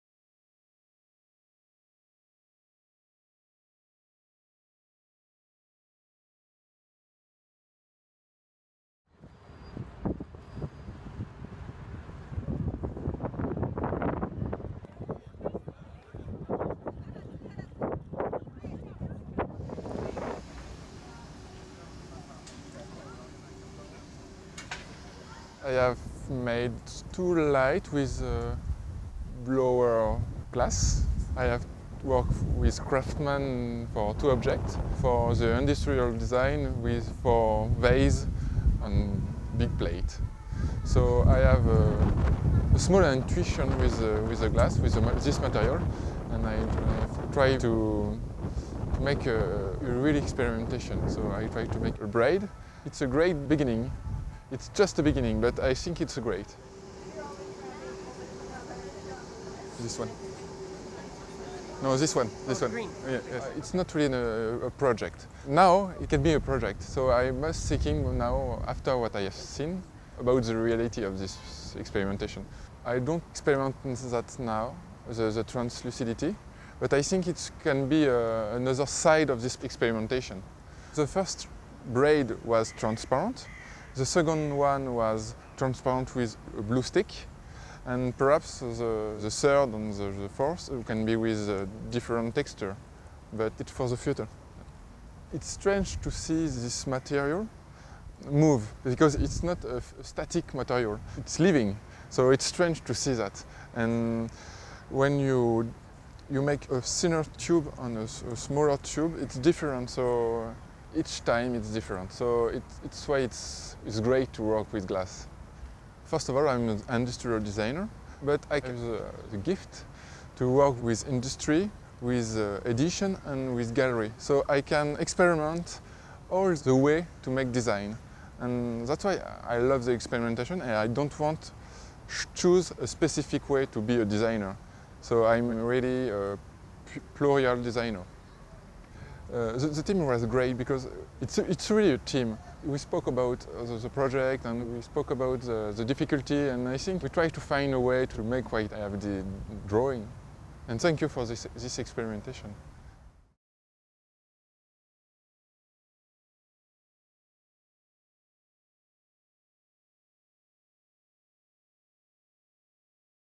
GlassLab in Paris: Benjamin Graindorge
The Corning Museum of Glass partnered with the Musée des Arts Décoratifs to present GlassLab, the Museum's design program, in Paris, in the Tuileries Garden, October 22-27, 2013.
French designer Benjamin Graindorge explored braiding glass during his GlassLab design session.